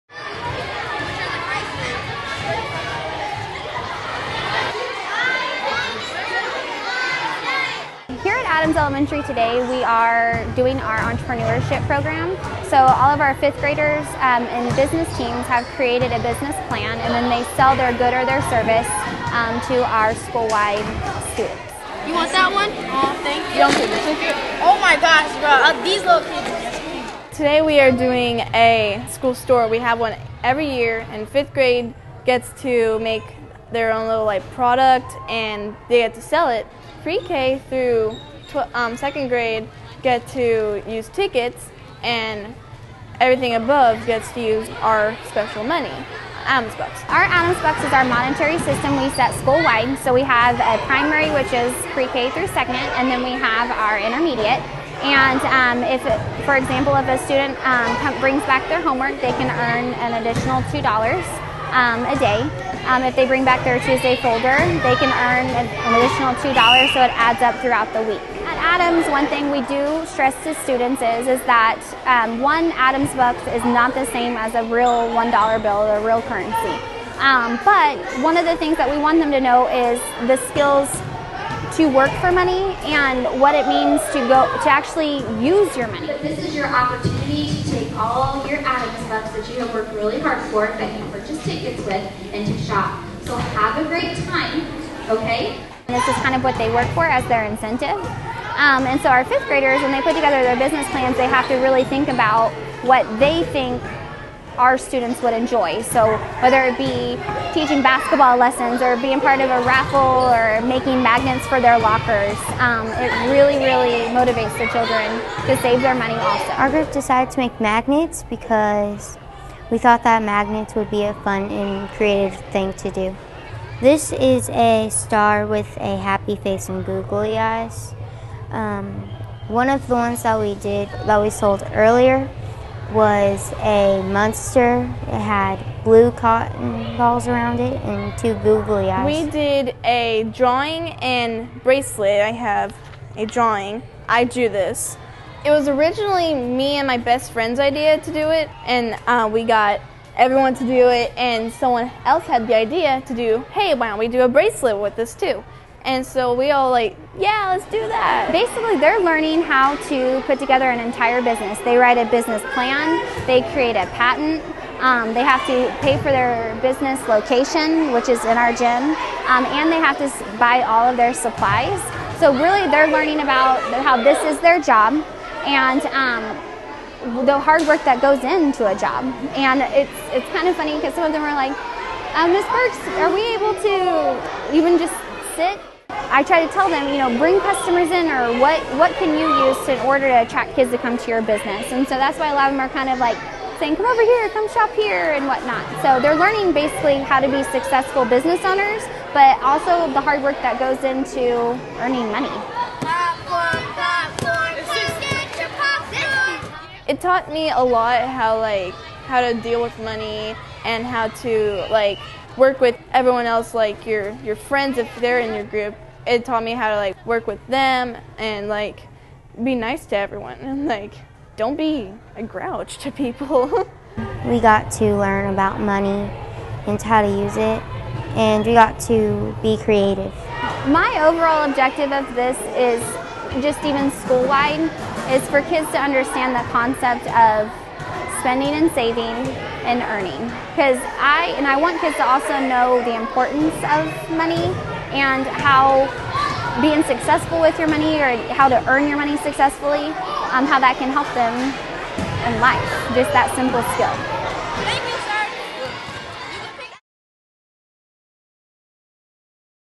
Adams School Store for Smart Money Week
Adams Elementary students celebrate Money Smart Week with their annual school store, in which fifth grade students set up mock businesses and sell their products and services to the rest of the student body using Adams Bucks as currency.